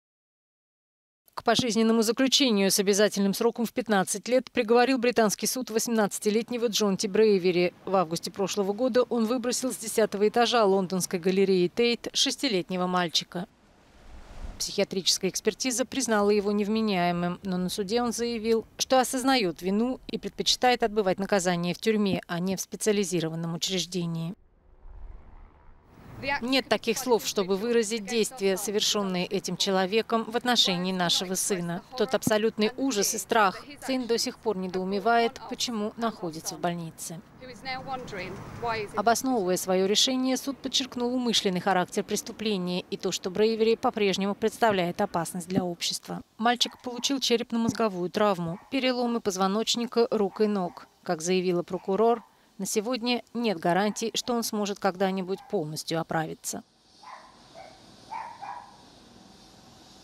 Пожизненный срок за покушение на убийство ребёнка
К пожизненному заключению с обязательным сроком в 15 лет приговорил британский суд 18-летнего Джонти Брейвери. В августе прошлого года он выбросил с десятого этажа лондонской галереи Тейт 6-летнего мальчика, который получил тяжелейшие травмы.